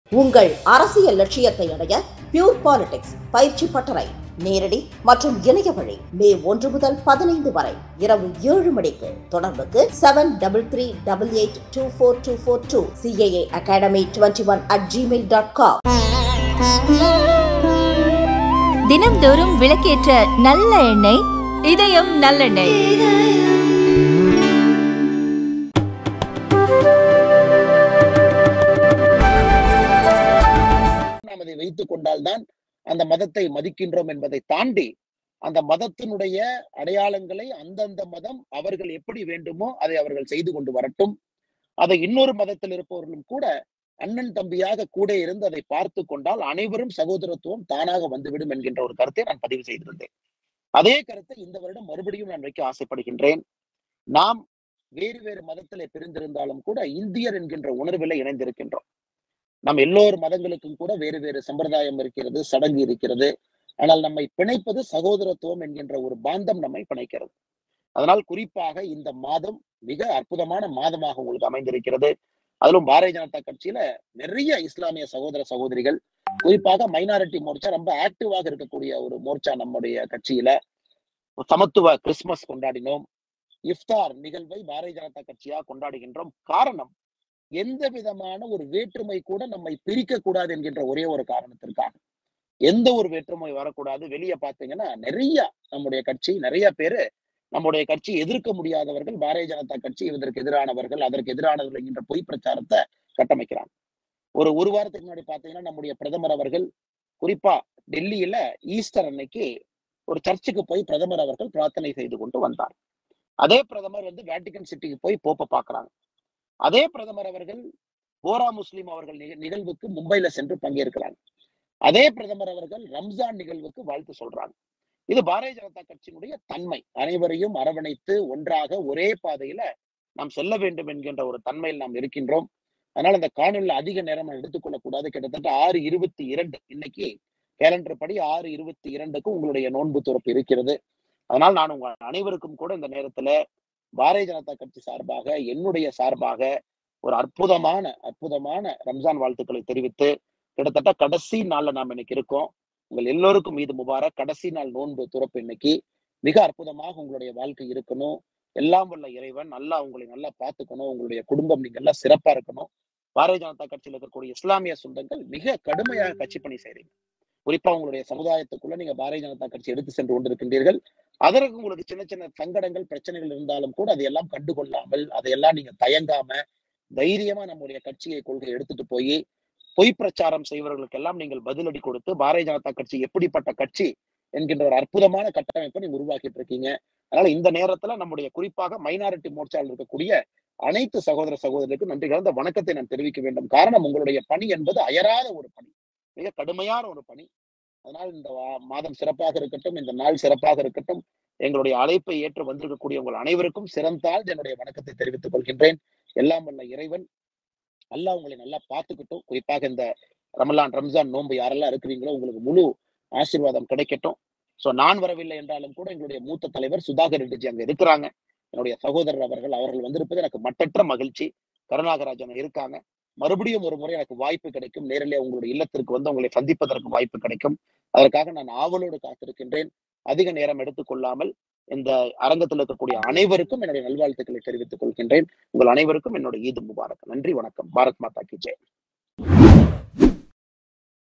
Annamalai Latest speech about Hindu Muslim unity | Bjp
சாணக்யா!